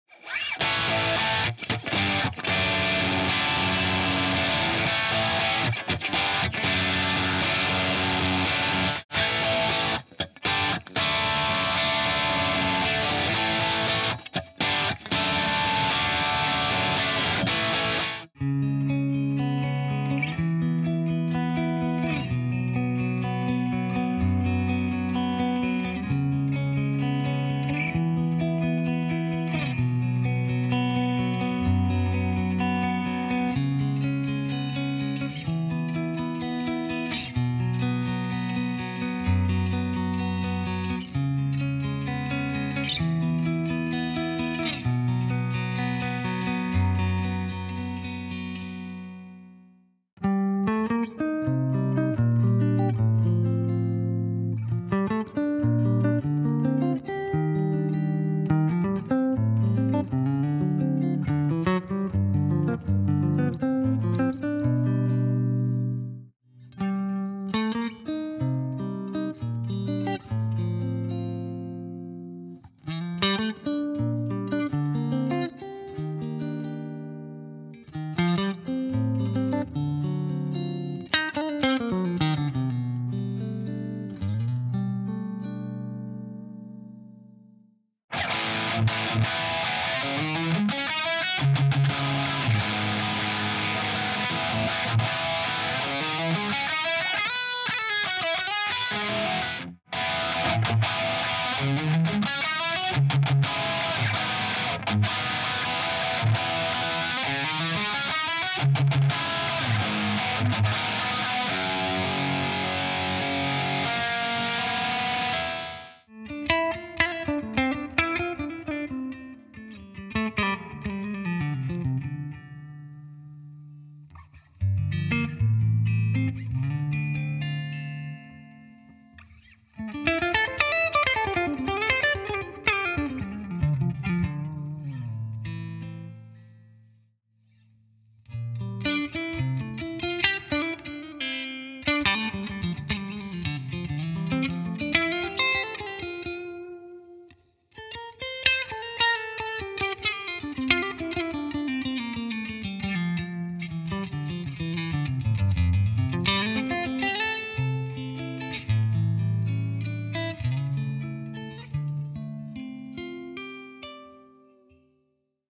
Sälen Jazz VS Boden Classic (Sound Comparison)
Sound comparison of a Strandberg Sälen Jazz VS a Strandberg BODEN CLASSIC

0:00 Salen Jazz / British Dirt (Bridge Pickup)
0:09 Boden Classic / British Dirt (Bridge Pickup)
0:18 Salen Jazz / Clean  (Neck Pickup)
0:33 Boden Classic / Clean  (Neck Pickup)
0:50 Salen Jazz / Jazz Pure (Neck Pickup)
1:07 Boden Classic / Jazz Pure (Neck Pickup)
1:28 Salen Jazz / Plini Style (Bridge Pickup)
1:40 Boden Classic / Plini Style (Bridge Pickup)
1:56 Salen Jazz / Clean solo (Neck Pickup)
2:18 Boden Classic  / Clean solo (Neck Pick)

 I am using BIAS from positive Grid for the tones.